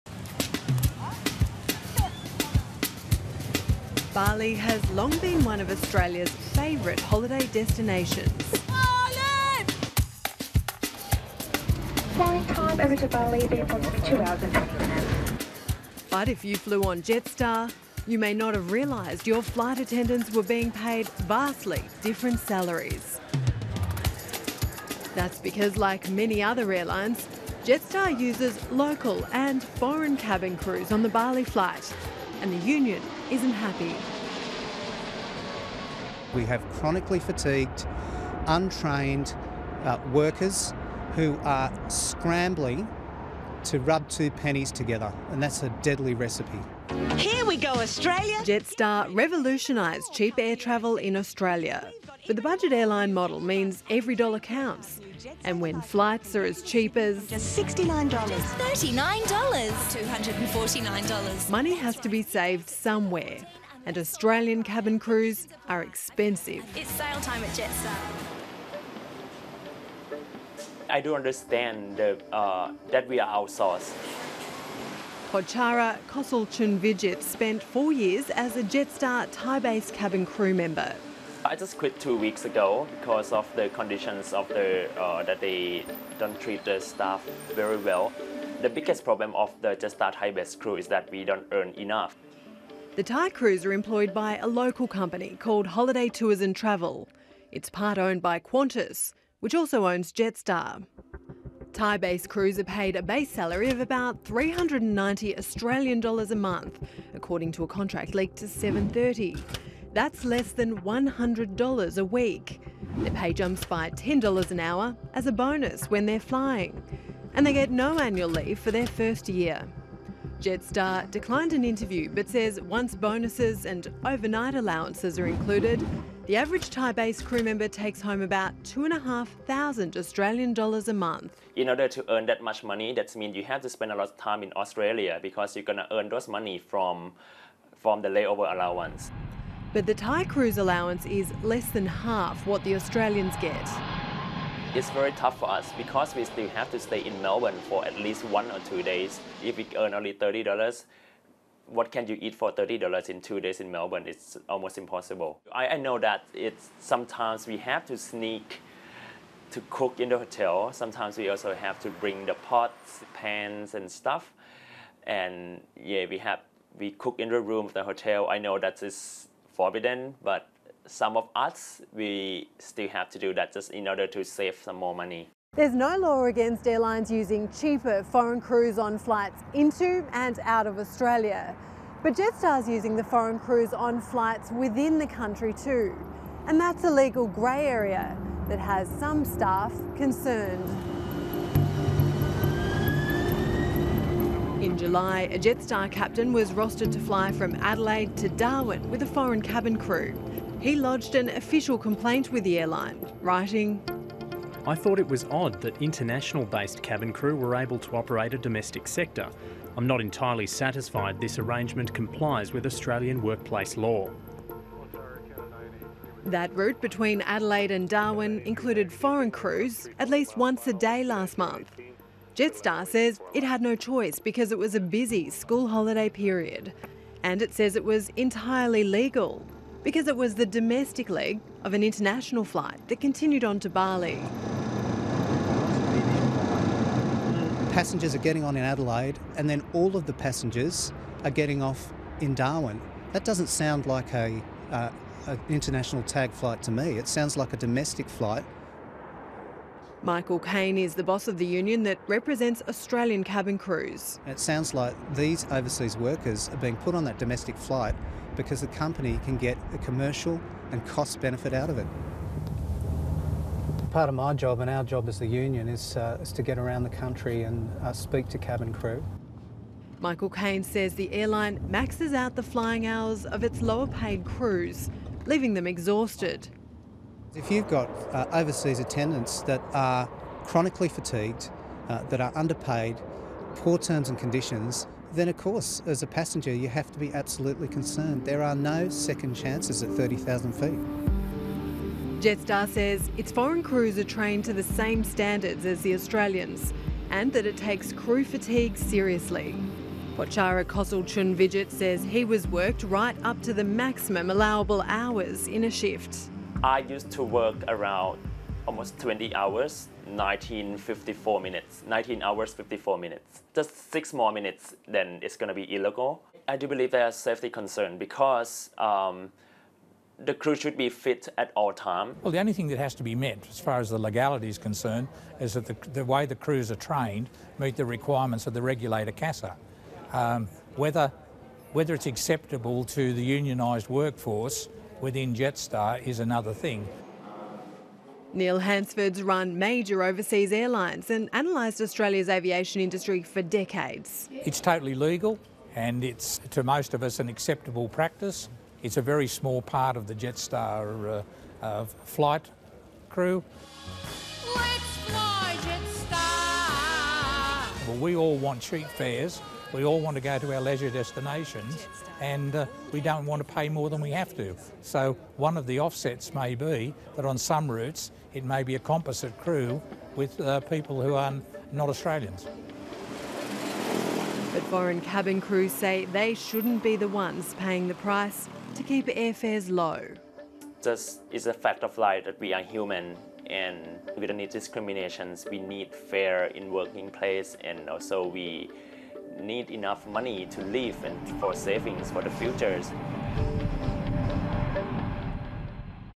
Jetstar Caught Out Ripping Off Thai Based Cabin Crew Paying Only $100 Week
Budget airline Jetstar is paying its Thai cabin crew a base salary of as little as $100 a week.